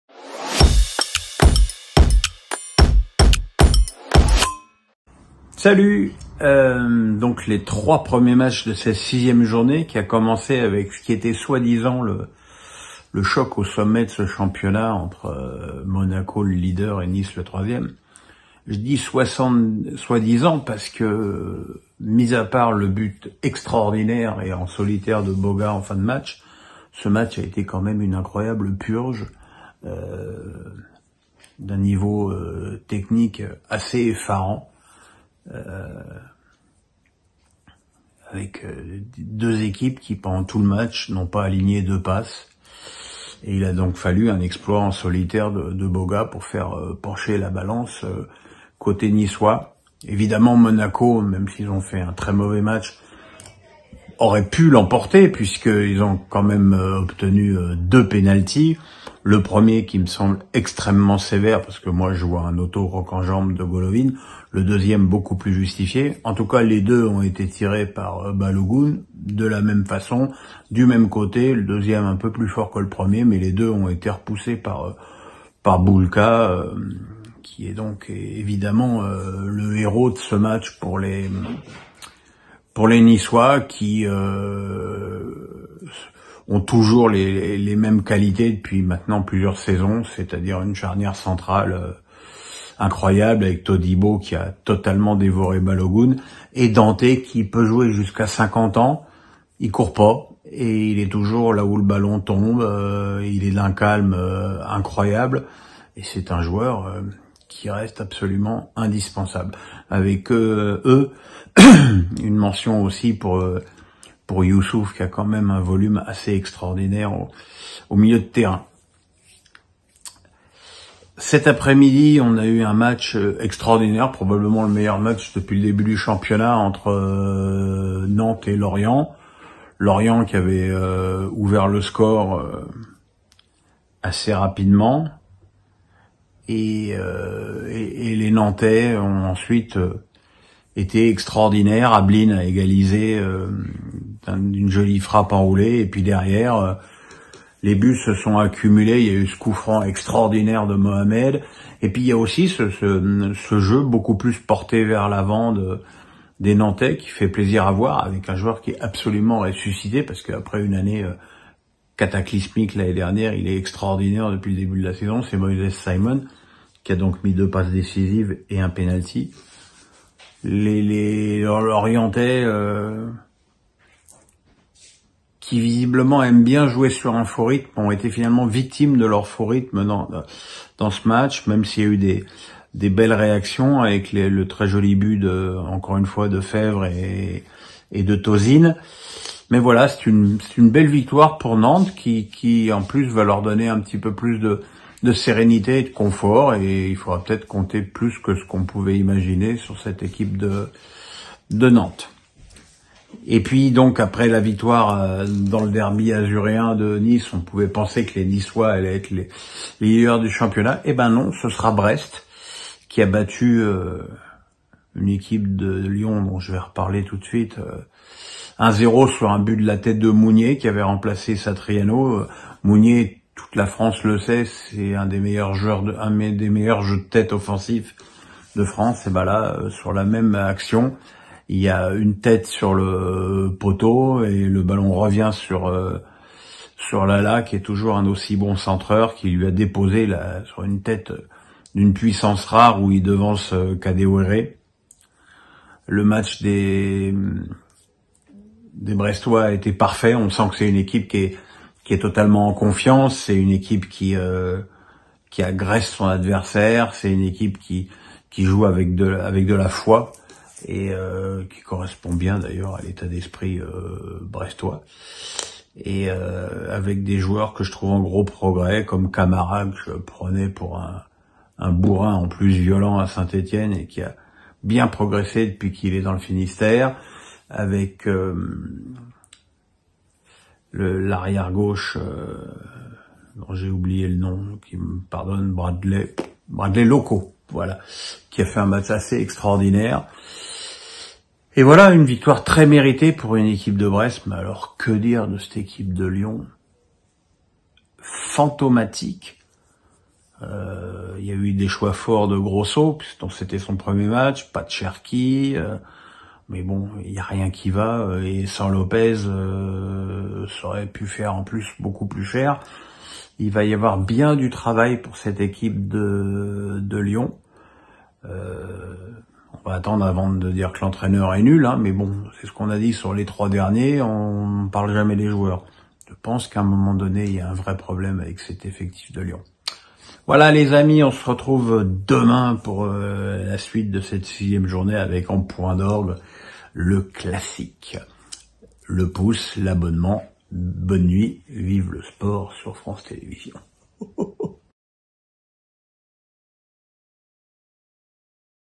Ligue 1, J6 (1) : Nice piège Monaco, Nantes fait le show, Brest accable Lyon
Allez, on débriefe les 3 premiers matchs de cette 6e journée.